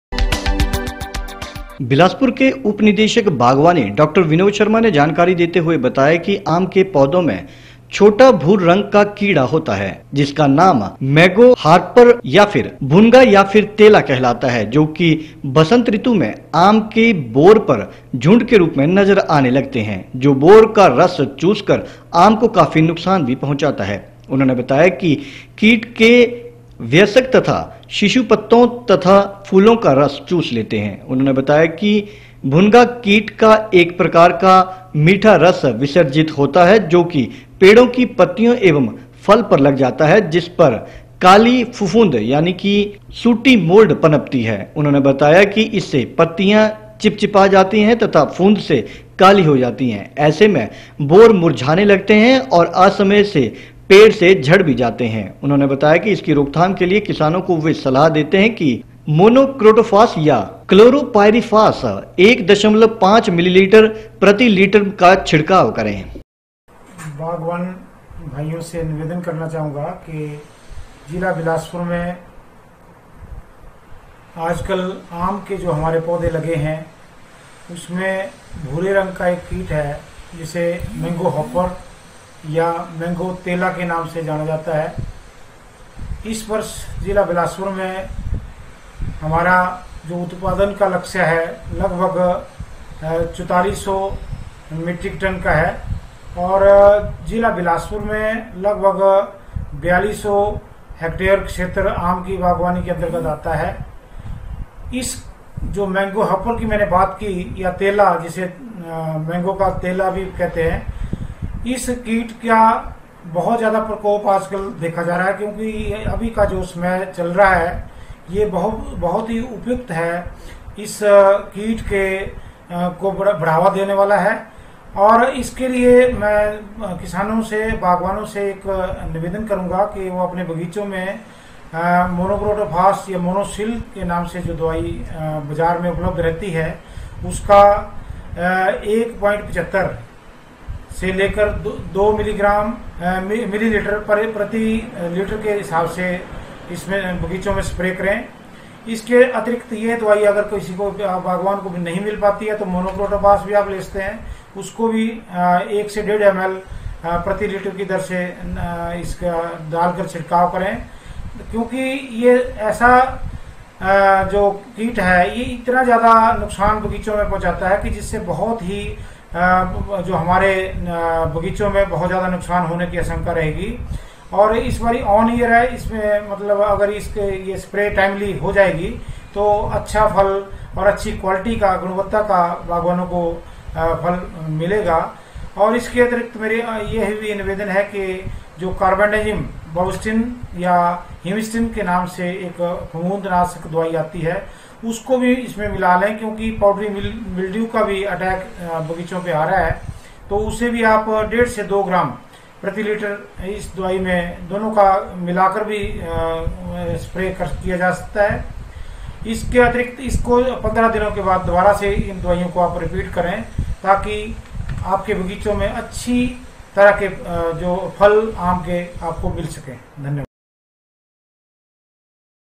23 may 3 आम के पौधों पर मोनोक्रोटोफास या क्लोरोपाइरिफास का छिड़काव करें
आम के पौधों पर मोनोक्रोटोफास या क्लोरोपाइरिफास का छिड़काव करें
आमों के पौधों में पाया जाता है भूरे रंग का कीड़ा
उप-निदेशक बागवानी डाॅ. विनोद शर्मा ने जानकारी देते
हुए बताया कि आम के पौधों में एक छोटा भूरे रंग का कीट होता है जिसका नाम मैंगो हाॅपर/भुनगा/तेला है। जोकि वसंत ऋतु में आम की बौर पर झुंड के रूप में नजर आने लगते हैं जो बौर का रस चूसकर आम को काफी नुकसान पहंुचाते है। उन्होंने बताया कि कीट के वयस्क तथा शिशु पत्तों तथा फूलों का रस चूसते
है।